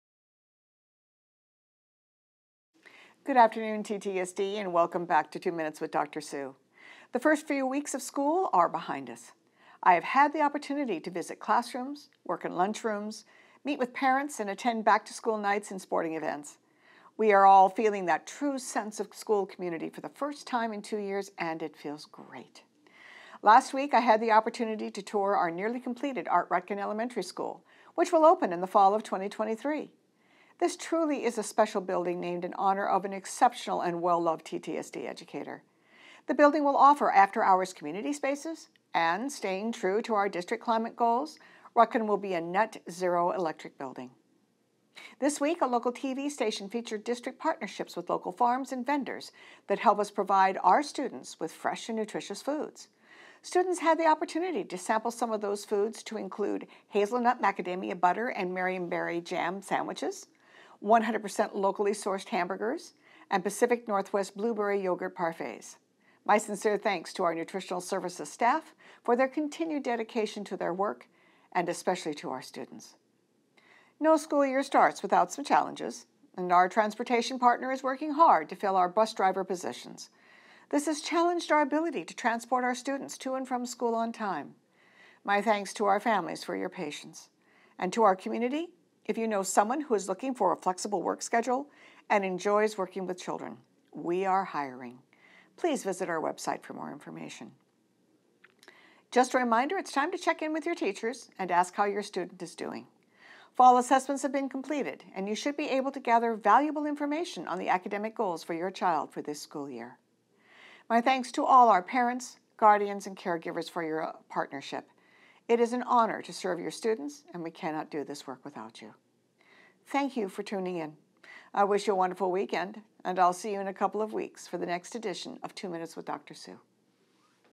2 With Dr. Sue - October 7th, 2022
Video Script

Good afternoon, TTSD, and welcome back to two minutes with Dr. Sue.
The first few weeks of the school year are behind us. I have had the opportunity to visit classrooms, work in lunch rooms, meet with parents, and attend back-to-school nights and sporting events. We are all feeling that true sense of school community for the first time in two years and it feels great!
Last week I had the opportunity to tour our nearly completed Art Rutkin Elementary School, which will open in the Fall of 2023. This truly is a special building named in honor of an exceptional and well-loved TTSD educator. The building will offer after hours community spaces and staying true to our district climate goals, Rutkin will be a net-zero electric building.
This week, a local TV station featured district partnerships with local farms and vendors that help us provide our students with fresh and nutritious foods. Students had the opportunity to sample some of those foods to include hazelnut macadamia butter and marionberry jam sandwiches; 100% locally sourced Hamburgers, and Pacific NW Blueberry Yogurt Parfaits. My sincere thanks to our nutrition services staff for their continued dedication to their work and especially to our students.
No school year starts without some challenges. Our transportation partner is working hard to fill our bus driver positions. This has challenged our ability to transport our students to and from school on time. My thanks to our families for your patience. And to our community – if you know someone who is looking for a flexible work schedule and enjoys working with children, we are hiring.
Just a reminder to check in with your teachers and ask how your student is doing. Fall assessments have been completed and you should be able to gather valuable information on the academic goals for your child for this school year.
My thanks to all our parents, guardians, and caregivers for your partnership. It is an honor to serve your students and we cannot do this work without you.
Thank you for tuning in. I wish you a wonderful weekend and I’ll see you in a couple of weeks for the next edition of two minutes with Dr. Sue.